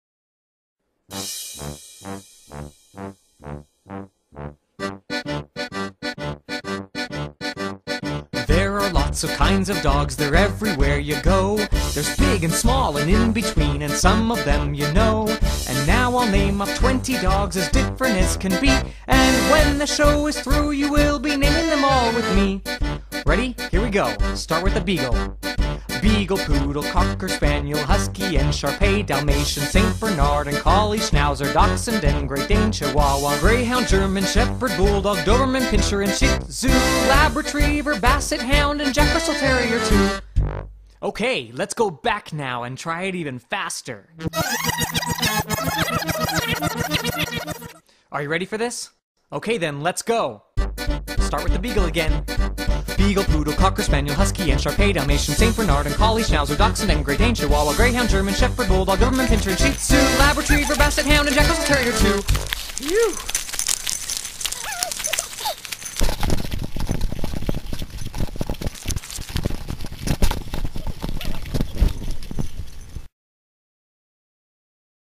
Funny dogs Leon & Srecko
There are two amazing dogs named Leon,he is gold retriver and Srecko,he is a tibetan spaniel.
They are so funny.